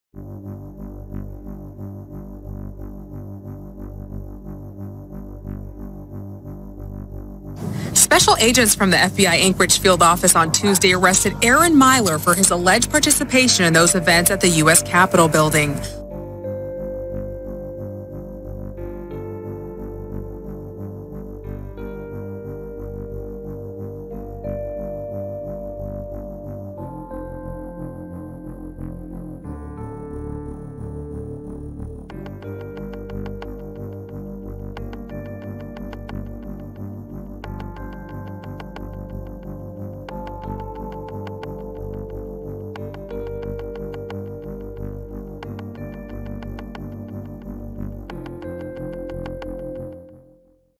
US Capitol Arrests: Aaron Mileur
Aaron Mileur was arrested for his participation in the January 6, 2021 attack and attempted insurrection at the United States Capitol.